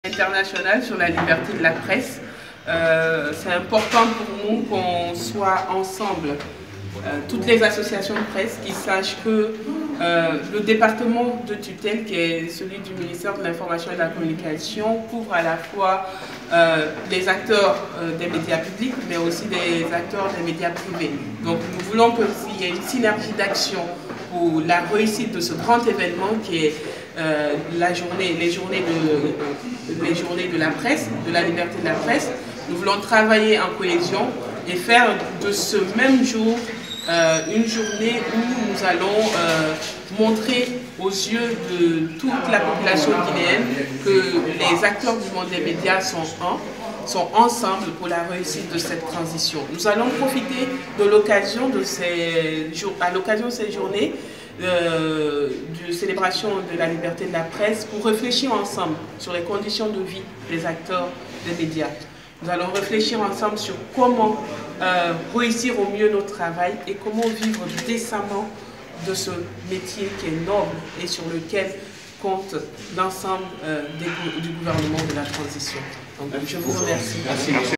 Préparatifs du 3 mai : le ministère de la communication échange avec les associations de presse
C’est parti pour les préparatifs de la journée internationale de la liberté de la presse, prévue le 3 mai prochain. La première réunion préparatoire de cet événement a eu lieu ce vendredi, 11 mars 2022, à Conakry. Elle a réuni des du ministère de l’information et de la communication et des représentants des associations de presse du pays. Les discussions ont porté sur l’organisation de l’événement et les activités à mener dans ce cadre, a appris Guineematin.com à travers un de ses journalistes.

Cette année, le ministère de l’information et de la communication compte organiser la célébration de la journée internationale de la liberté de la presse de façon concertée, avec la participation de tous les acteurs concernés. C’est dans ce sens que le département a organisé cette première réunion préparatoire avec l’ensemble des associations de presse du pays. Selon la ministre, Rose Pola Pricémou, l’objectif est de créer une synergie d’actions pour la réussite de cet événement annuel, dédié à la liberté de la presse.

« Le 3 mai, c’est la journée internationale de la liberté de la presse. C’est donc important pour nous d’être ensemble avec toutes les associations de presse. Qu’elles sachent que le département de tutelle couvre à la fois les acteurs des médias publics et ceux des médias privés. Donc, nous voulons qu’il y ait une synergie d’actions pour la réussite de ce grand événement, qui est la célébration de la journée de la liberté de la presse.

Nous voulons travailler en cohésion pour faire de ce même jour une journée où nous allons montrer aux yeux de toute la population guinéenne que les acteurs du monde des médias sont un, sont ensemble, pour la réussite de cette transition. Nous allons profiter de l’occasion pour réfléchir ensemble sur les conditions de vie des acteurs des médias. Nous allons réfléchir sur comment réussir au mieux notre travail et comment vivre décemment de ce métier noble, sur lequel compte l’ensemble des membres du gouvernement », a déclaré la ministre de l’information et de la communication.

Cette rencontre préparatoire a permis aux participants d’identifier plusieurs actions à mettre en œuvre dans le cadre de la célébration de la journée internationale de la liberté de la presse. Une initiative vivement saluée par Amadou Tham Camara, le président de l’association guinéenne de la presse en ligne (AGUIPEL). Il estime que cette rencontre est très importante pour la presse guinéenne.

« Pour une fois, nous avons voulu que ça soit une fête inclusive, c’est-à-dire qu’il n’y ait pas de distinguo entre la presse publique et celle privée. Que tout le monde soit associé à la fête de sorte qu’au sortir de cela, il y ait des travaux importants en amont, notamment les propositions qui doivent être faites pour l’amélioration de la loi sur la liberté de la presse et les commissions qui vont être mises en place. C’est important, parce que vous n’êtes pas sans savoir qu’il y a de nouveaux médias qui ne sont pas dans cette loi-là. Également, il y a des dispositions qui doivent être corrigées.

Donc, avant le 3 mai, c’est de travailler sur comment améliorer notre loi. Ainsi, nous aurons un bon projet de loi que nous allons transmettre au gouvernement, en suite au CNT pour son adoption. Il y a aussi la convention collective que nous devons travailler avant le 3 mai pour que cette convention collective, qui va être avantageuse pour les employés mais également pour les employeurs, soit finalisée. Donc, c’était une rencontre très importante pour la presse à mon sens », a dit Amadou Tham Camara.

Selon le président de l’AGUIPEL, des formations et des panels seront animés le 3 mai. D’autres activités sont également prévues dans certaines universités pour célébrer la journée internationale de la liberté de la presse en Guinée.